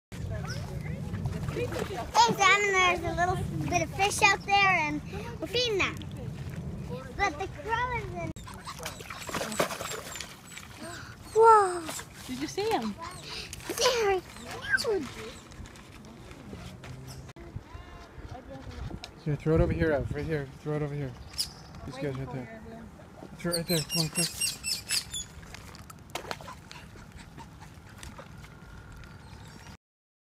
Graeme Hall Sanctuary, Barbados
Feeding the fish and ducks at Graeme Hall Sanctuary in Barbados